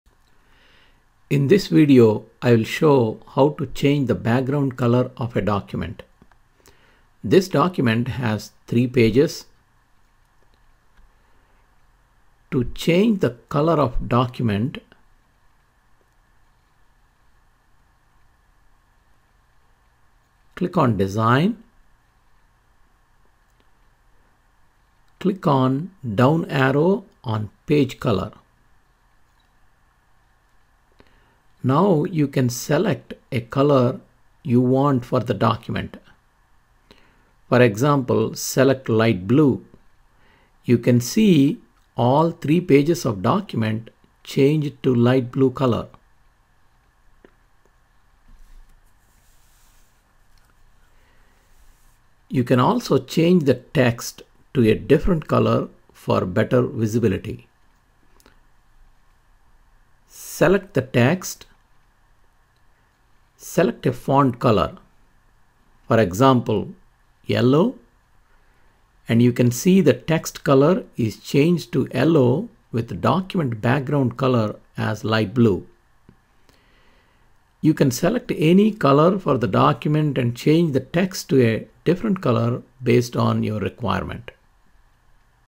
How to Change Back ground color of a document in Word 2016
You can change background color of a document, Microsoft Word allows you to select a color from color palette. You can change the text in the document to a different color for better visibility after you changed the background color of the document.